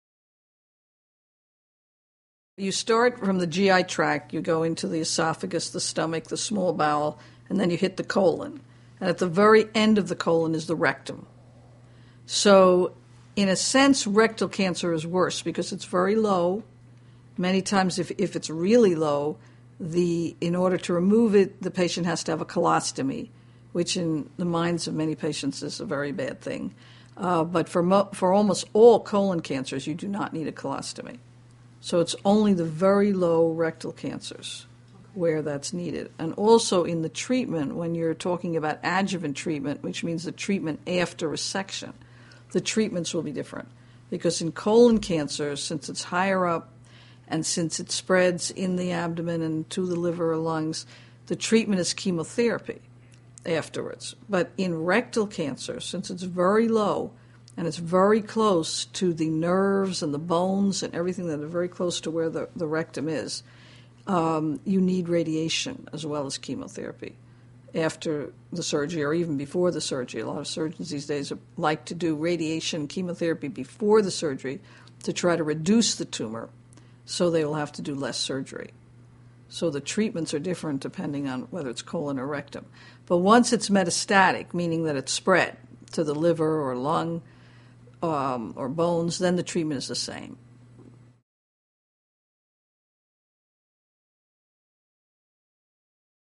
Colon vs Rectal Cancer
Colorectal cancer specialist Nancy Kemeny, MD, explains the differences between colon and rectal cancers.

You start from the GI tract, you go into the esophagus, the stomach, the small bowel, and then you hit the colon. And at the very end of the colon is the rectum. So, in a sense, rectal cancer is worse, because it's very low, many times if it's really low, in order to remove it, the patient has to have a colostomy, which in the minds of many patients is a very bad thing. But for almost all colon cancers, you do not need a colostomy. So it's only the very low rectal cancers where that's needed, and also in the treatment, when you're talking about adjuvant treatment, which means the treatment after a section, the treatments will be different.

Because in colon cancer, since it's higher up, and since it spreads in the abdomen, and to the liver and lungs, the treatment is chemotherapy afterwards. But in rectal cancer, since it's very low, and it's very close to the nerves and the bones and everything is very close to where the rectum is, you need radiation as well as chemotherapy after the surgery, or even before the surgery. A lot of surgeons these days like to do radiation and chemotherapy before the surgery to try to reduce the tumor, so they will have to do less surgery.

So the treatments are different, depending on whether it's colon or rectum. But once it's metastatic, meaning that it's spread to the liver or lungs, or to the bones, then the treatment is the same.